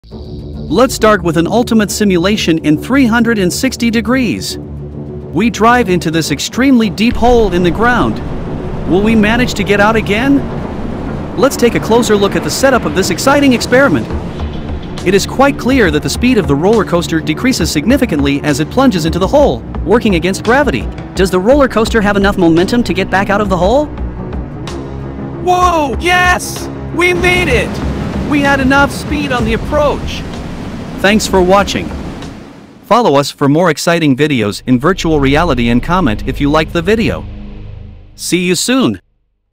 Train Vs Giant Pit in 360° (MrBeast Challenge)
Let's start with an ultimate simulation. We drive into this extremely deep hole in the ground. Will we manage to get out again?

Let's take a close look at the framework/setup of this exciting experiment! It is quite clear that the speed of the roller coaster decreases significantly as it plunges into the hole, working against gravity.

Does the roller coaster have enough momentum to get back out of the hole?
Yay, we made it! We had enough speed on the approach.

Follow us for more exciting videos in virtual reality and comment if you liked the video. See you soon!

It is important to note that this simulation can not perform exact calculations to determine the exact probability of the roller coaster coming out. This requires more detailed technical analysis and consideration of physical laws and parameters. So it remains exciting and thrilling to see if the roller coaster has enough momentum to drive out of the hole again!